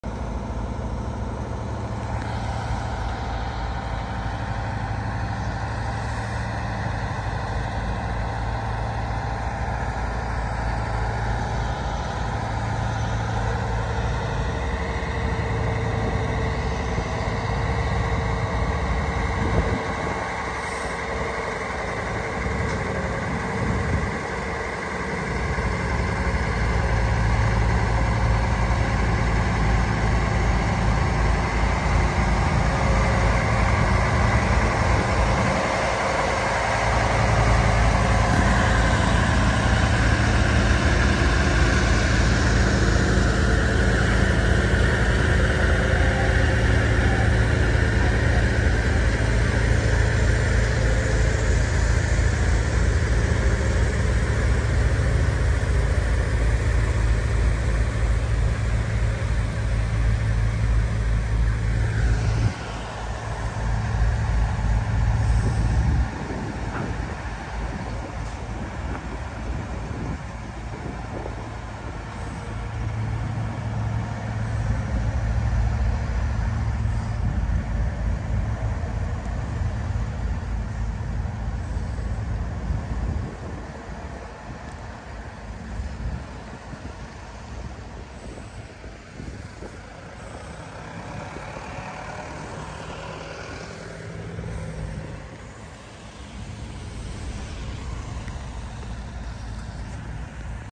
SCANIA 650S ブレード輸送のため、左折出来ない交差点を側道から国道に合流
高圧線があり、国道に出る交差点を左折出来ないため、側道を利用して国道に合流する場面です。
電材重機さんの新型スカニアXT 650SがGE社製の風車ブレードを取り付けたFTV550を牽き、夜間輸送しています。